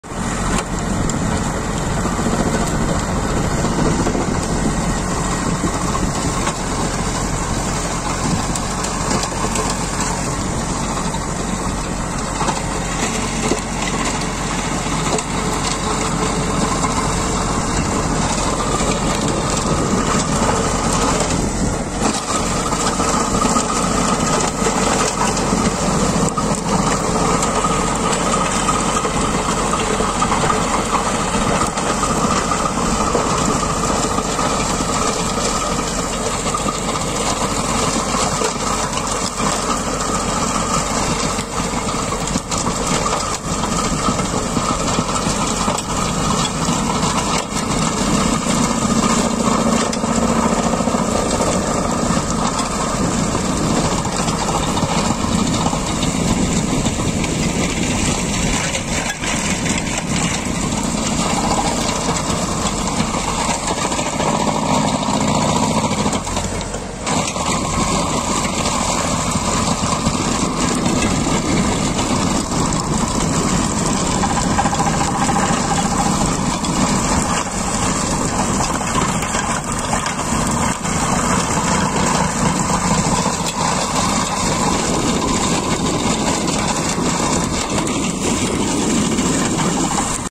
Taş kırma makinası - ELİBOL
Dikim Öncesi ve sonrası bahçe tarla aralarındaki taşları kırıp mekanizasyona uygun hale getirir bahçe ve tarladaki 15-20 cm çapındaki serbest taşları kırabilir emsal makinelere göre saatte 4 litre yakıt tasarufu sağlar.